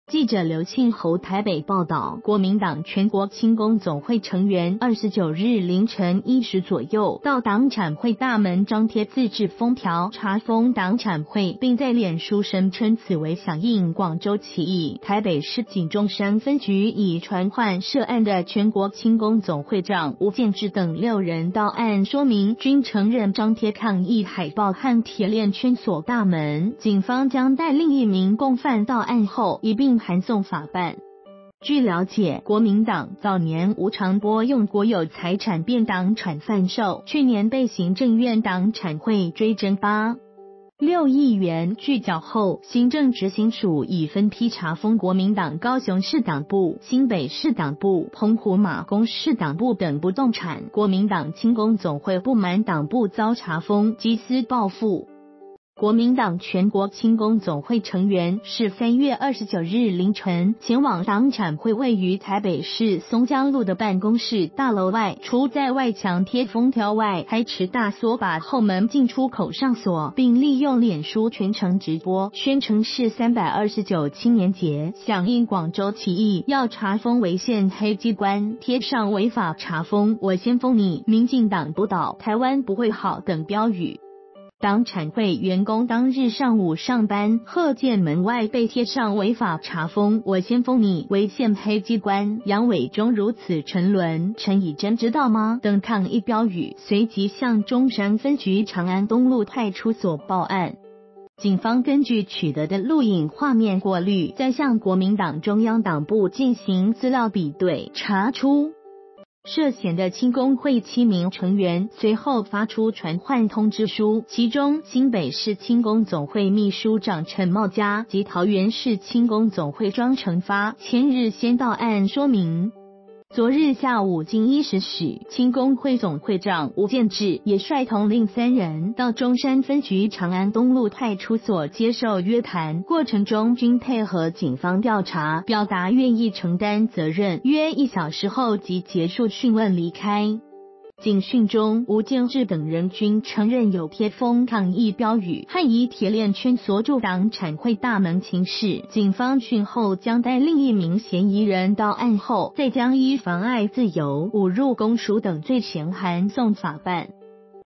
對黨產會貼封條鍊鎖大門 全國青工總會7人究辦
〔記者劉慶侯／台北報導〕國民黨全國青工總會成員，29日凌晨1時左右，到黨產會大門張貼自製封條「查封」黨產會，並在臉書聲稱此為「響應廣州起義」。台北市警中山分局已傳喚涉案的全國青工總會長吳建志等6人到案說明，均承認張貼抗議海報和鐵鍊圈鎖大門，警方將待另1名共犯到案後，一併函送法辦。

發表評論評論如下。